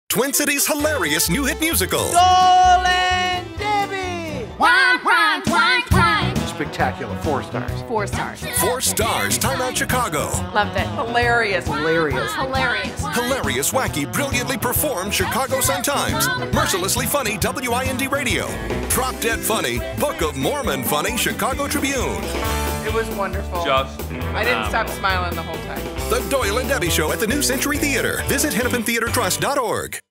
The Doyle & Debbie Show
Written by Minnesota's own Bruce Arntson, The Doyle & Debbie Show is the hilarious new musical comedy that the Chicago Tribune calls "Hilarious, drop-dead funny." The Doyle & Debbie Show lovingly pokes fun at country music has-been Doyle Mayfield and the lovely stars-in-her-eyes Debbie.